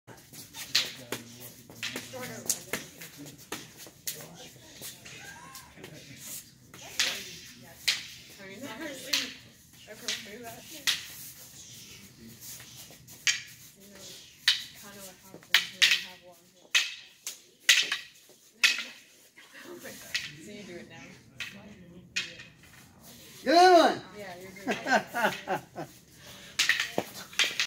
Kids Kali
Tyler learning 3 drills.